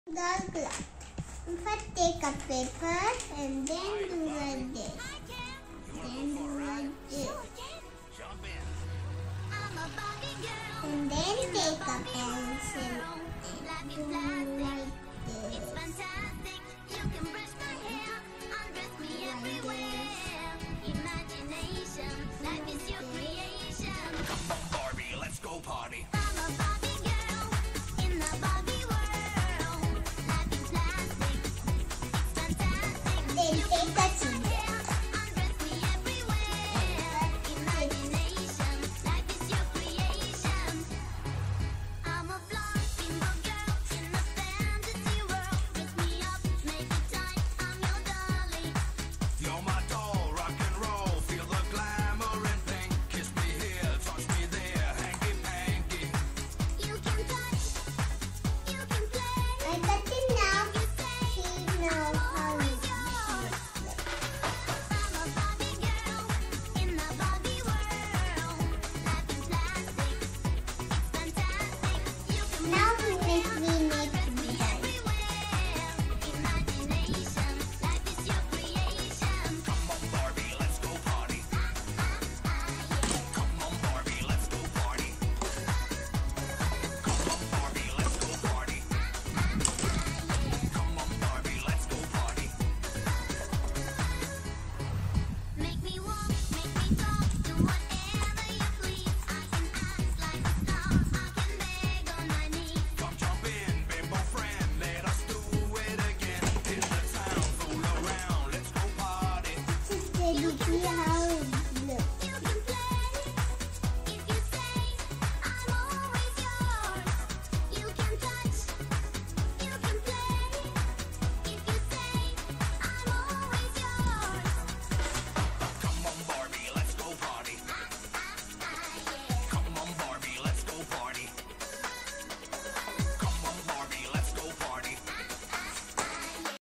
Fiona making doll clothes (tutorial) with Paper
Fiona makes paper clothes for doll on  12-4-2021, Monday.